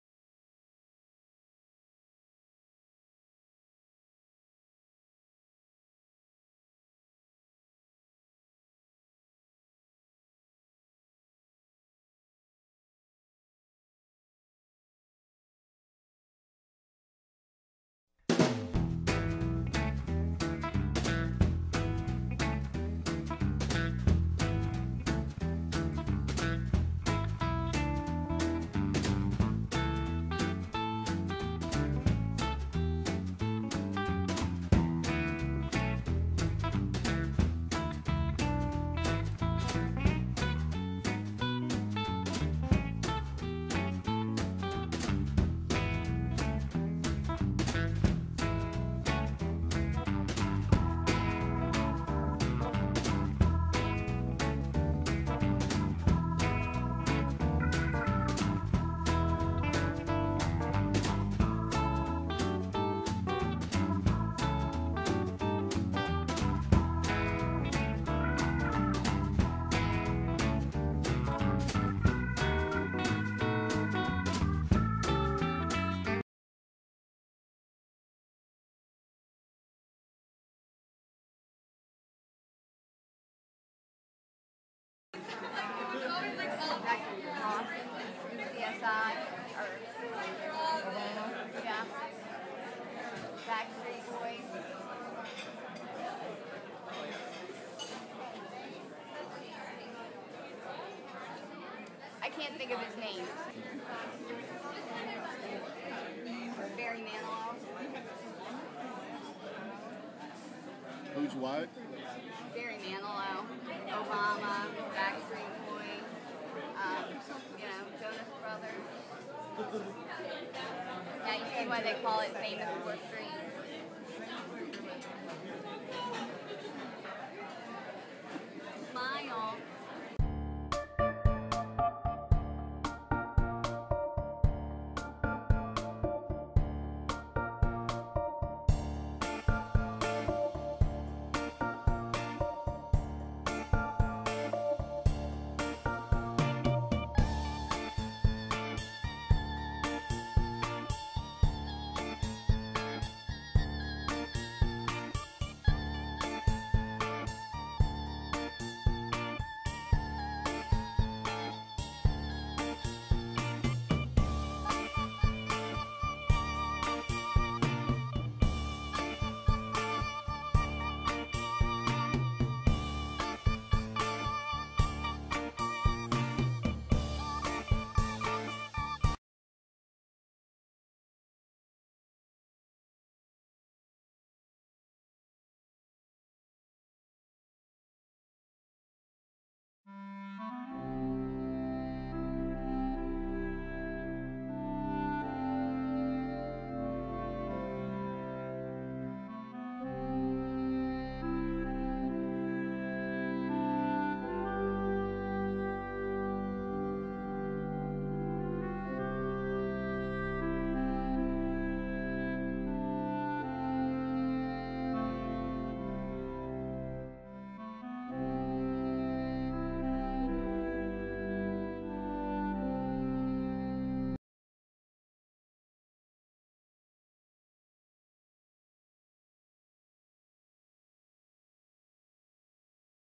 2nd Half of my Trip Nov. 6 - 9, 2011.wmv
This is the second half of my trip.  Family laid my brother in law to rest in Arlington National Cemetery.